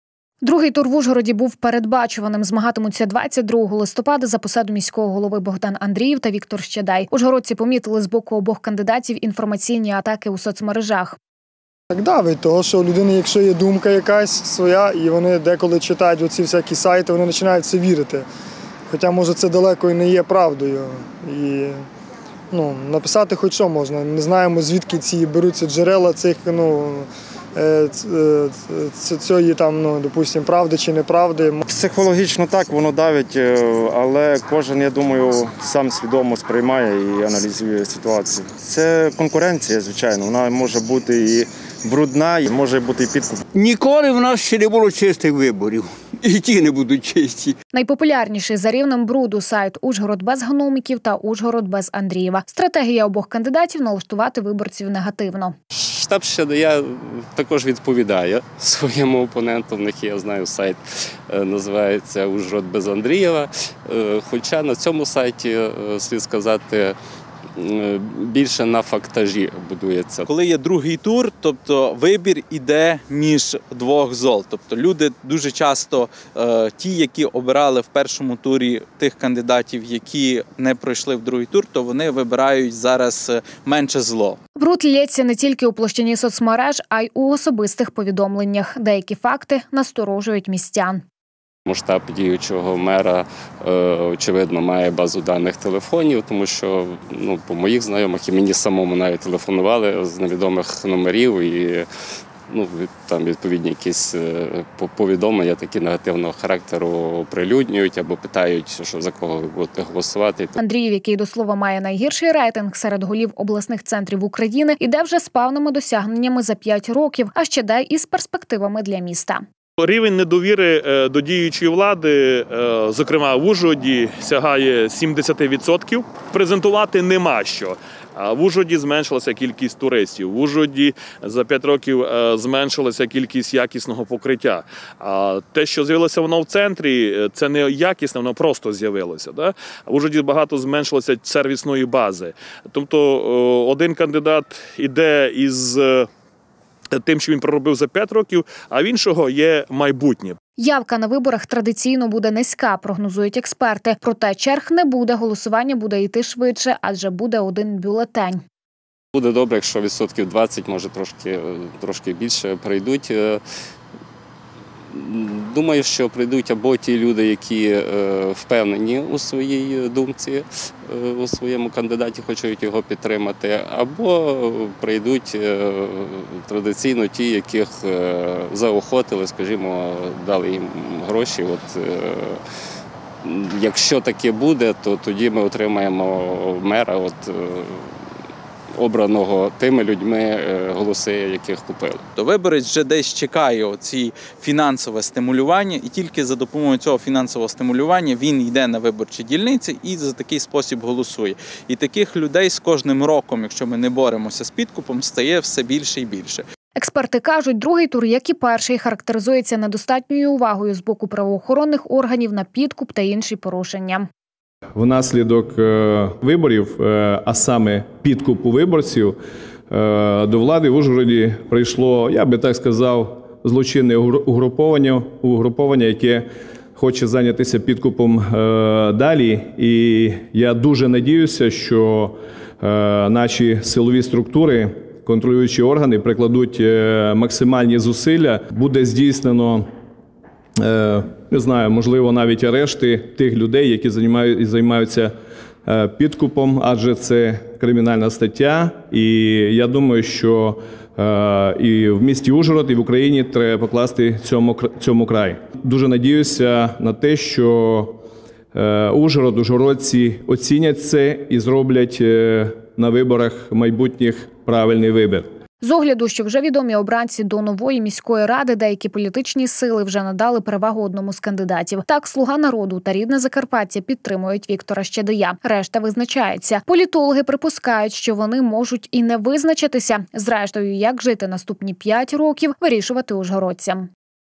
II тур. Вибори в Ужгороді
2 тур в Ужгороді був передбачуваним. Змагатимуться 22 листопада за посаду міського голови Богдан Андріїв та Віктор Щадей. Ужгородці помітили з боку обох кандидатів інформаційні атаки у соцмережах.
Найпопулярніші за рівнем бруду: сайт  «Ужгород без Гномиків» та «Ужгород без Андріїва». Стратегія обох кандидатів – налаштувати виборців негативно.
Бруд ллється не тільки у площині соцмереж, а й у особистих повідомленнях. Деякі факти насторожують містян.
Шанси обох кандидатів майже рівні. Андріїв, який, до слова, має найгірший рейтинг серед  голів обласних центрів України, іде вже з певними досягненнями за 5 років, а Щадей з перспективами для міста.
.Явка на виборах, традиційно, буде низька –прогнозують експерти. Проте черг не буде голосування буде іти швидше, адже буде один бюлетень.
 Каже: буде добре якщо 20 % прийдуть. Це ті хто визначивя з кандидатом. А також, кого зацікавили грошима. Якщо так буде – то тоді отримаємо мера, обраного людьми, яких підкупили
Експерти кажуть, 2 тур, як і 1 –ий, характеризується недостатньою увагою з боку правоохоронних органів на підкуп та інші порушення.
З огляду, що вже відомі обранці до нової міської ради, деякі політичні сили вже надали перевагу одному з кандидатів. Так, Слуга Народу та   Рідне Закарпаття підтримують Віктора Щадея, решта – визначаються. Політологи припускають, що вони можуть і не визначитися. Зрештою, як жити наступні 5 років – вирішувати ужгородцям.